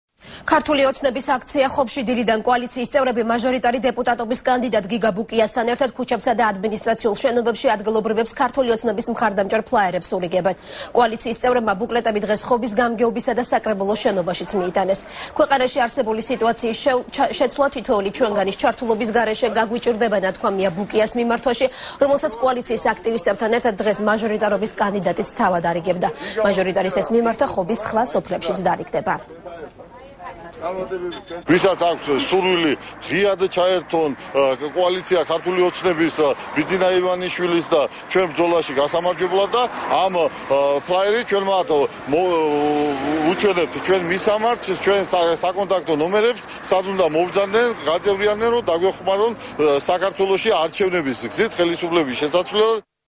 ახალი 6 | ოცნების აქცია | 13.08.12
ქართული ოცნების აქცია ხობში. დილიდან კოალიციის წევრები მაჟორიტერი დეპუტატობის კანდიდატ გიგა ბუკიასთან ერთად ქუჩებსა და ადმინისტრაციულ შენობებში ადგილობრივებს ქართული ოცნების მხარდამჭერ ფლაერებს ურიგებენ. კოალიციის წევრებმა ბუკლეტები დღეს ხობის გამგეობისა და საკრებულოს შენობაშიც დატოვეს. „ქვეყანაში არსებული სიტუაციის შეცვლა თითოეული თქვენგანის ჩართულობის გარეშე  გაგვჭირდება," ნათქვამია გიგა ბუკიას მიმართვაში, რომელსაც კოალიციის აქტივისტებთან ერთად მჟორიტარობის კანდიდატი თვითონ არიგებდა. მაჟორიტარის ეს მიმართვა ხობის სოფლებშიც დარიგდება.